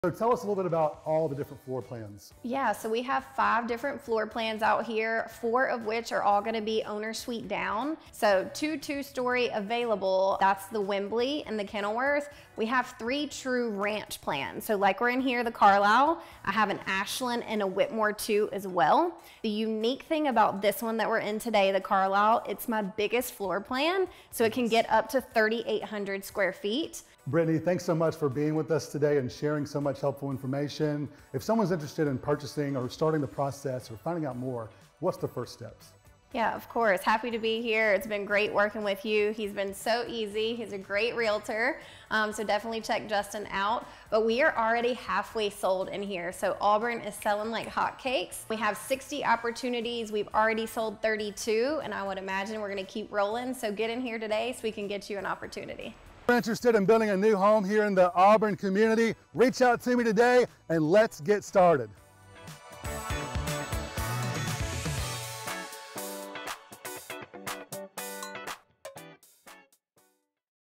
Walkthrough at ALLBURN with Justin Porter - Part 4
NEW CONSTRUCTION is HOT, HOT, HOT!! Let's go to CONCORD! Taylor Morrison's ALLBURN Community located in Concord is one of the hottest around.

Full Allburn Walkthrough VLOG below!

Ready to take a tour and possibly start the process? Justin is ready to help you start today! Reach out now!